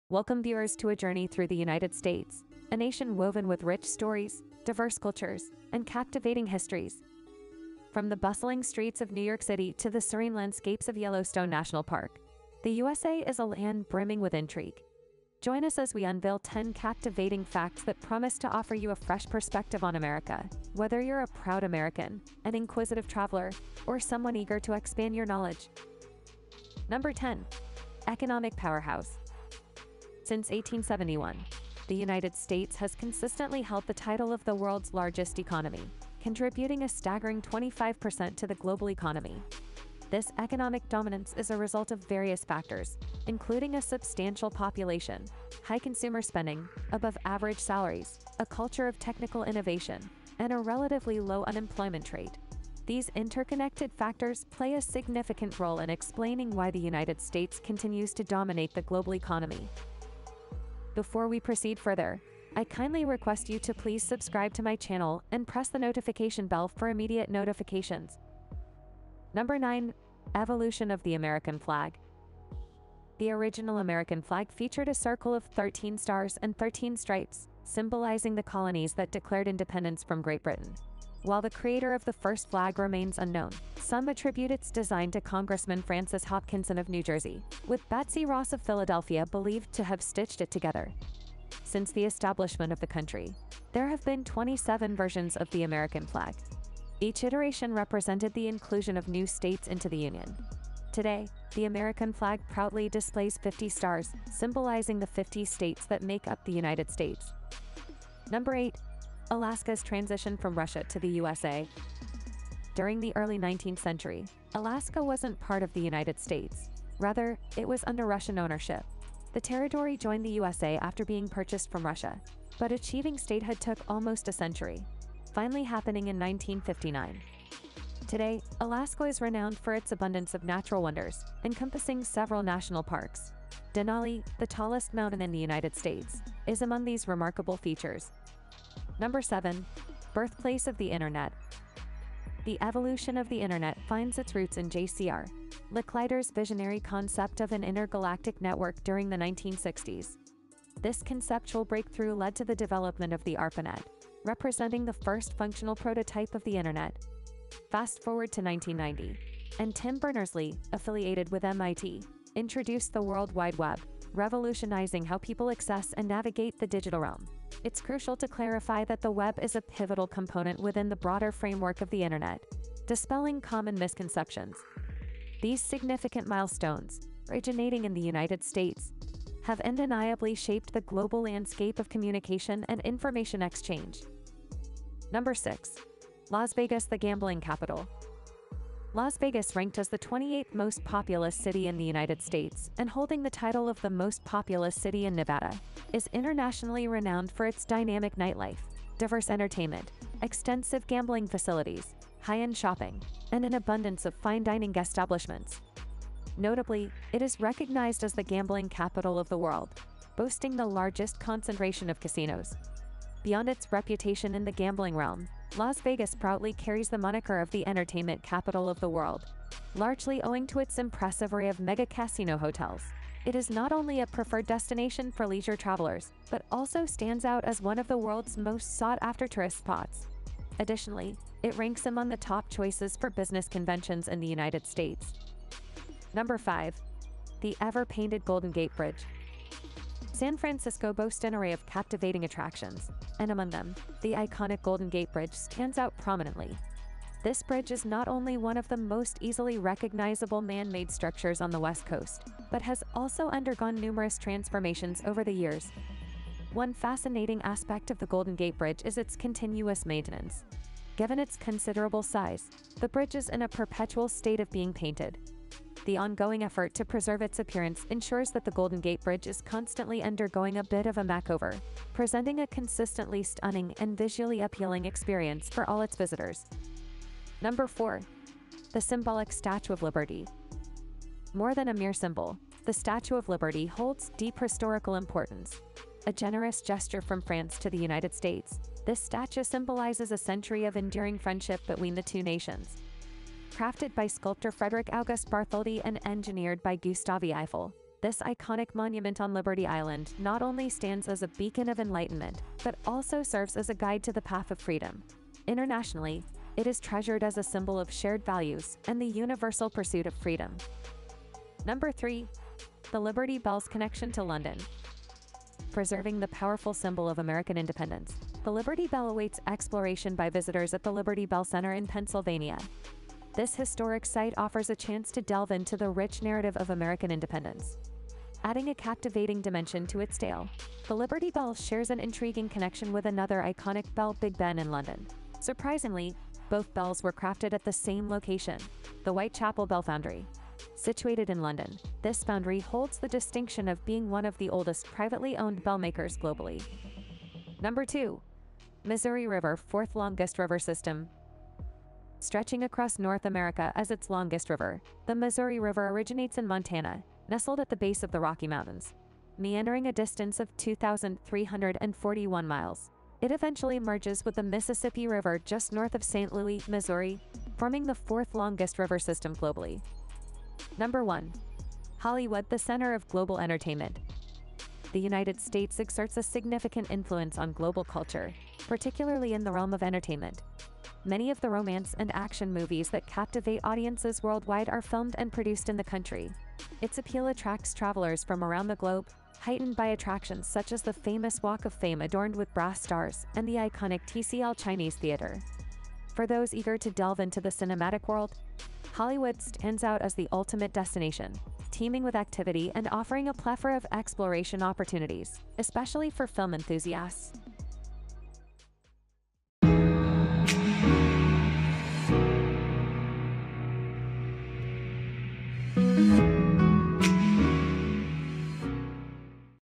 Top 10 Facts About America You Need To Know
Welcome viewers, to a journey through the United States, a nation woven with rich stories, diverse cultures, and captivating histories. From the bustling streets of New York City to the serene landscapes of Yellowstone National Park, the USA is a land brimming with intrigue. Join us as we unveil 10 captivating facts that promise to offer you a fresh perspective on America, whether you're a proud American, an inquisitive traveler, or someone eager to expand their knowledge.

Did You Find These Top 10 Facts About America You Need To Know interesting? Tell us which one surprised you the most in the comments below!